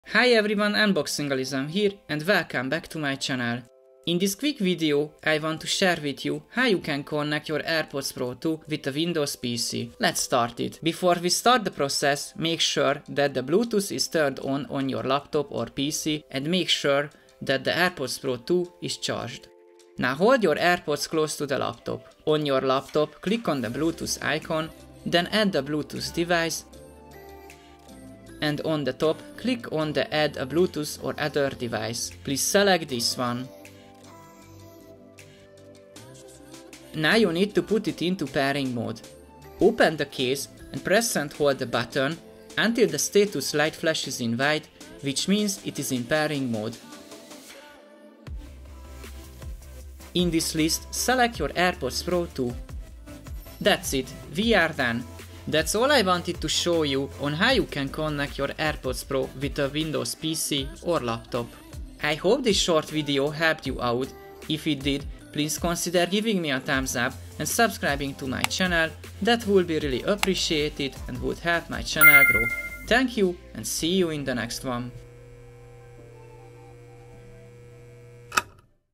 How To Connect AirPods Pro 2 To Windows PC / Laptop
The How To Connect AirPods Pro 2 To Windows PC / Laptop video finally arrived to the channel!
In this short video I will show you ho you can connect your AirPdos Pro 2 with a windows PC.

00:00 - Intro
00:10 - Before You start it
00:20 - How To Connect

I have made more Series 8 video as well.

I hope you will enjoy this video, and I hope you will learn something new.